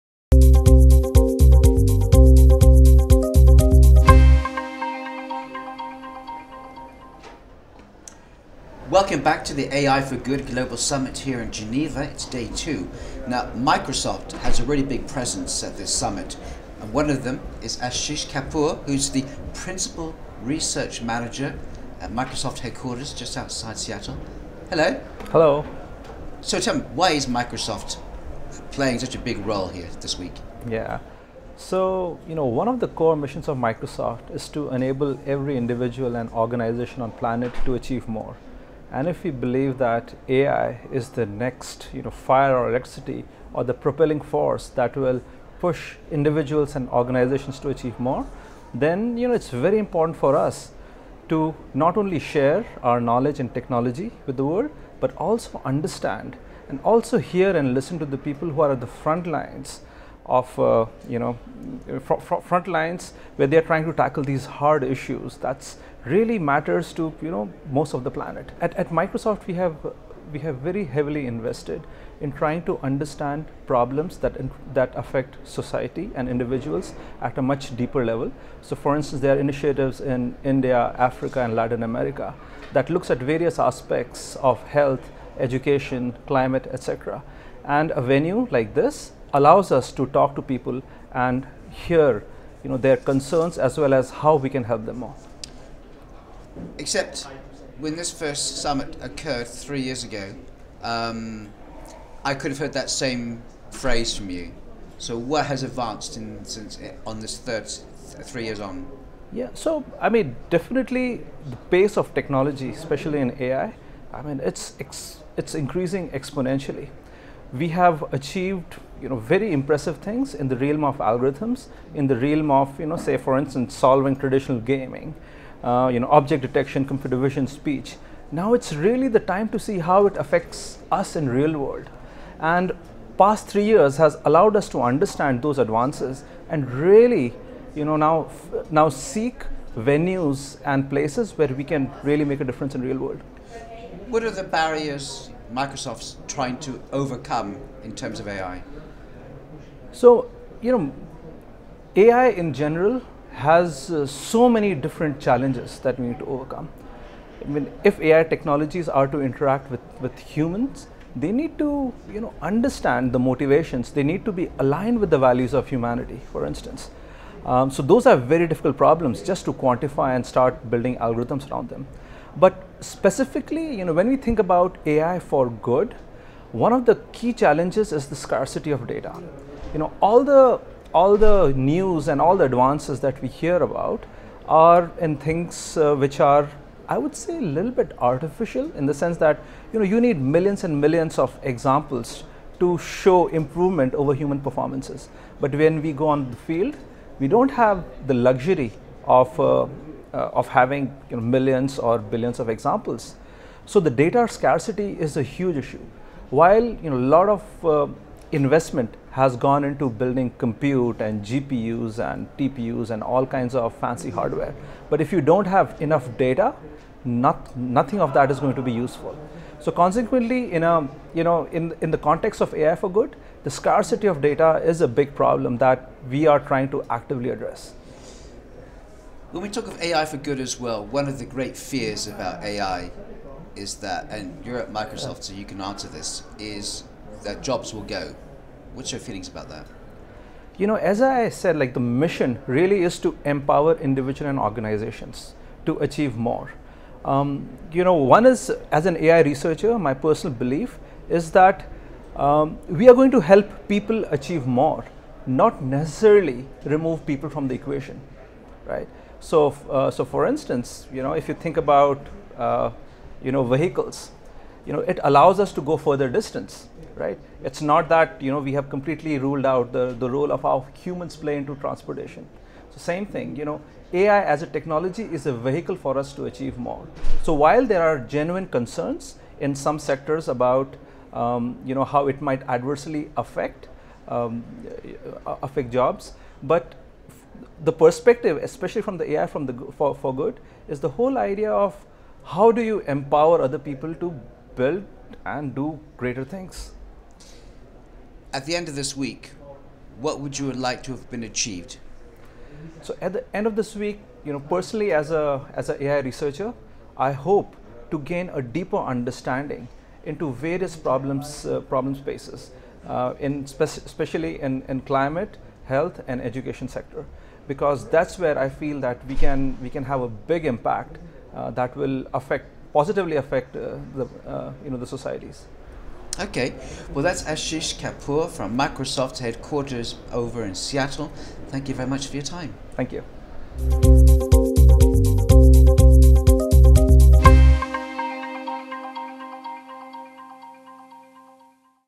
AI FOR GOOD 2019 INTERVIEWS: Ashish Kapoor, Principal Research Manager, Microsoft
Interview with Ashish Kapoor, Principal Research Manager at Microsoft at the AI for Good Global Summit 2019, ITU, Geneva, Switzerland.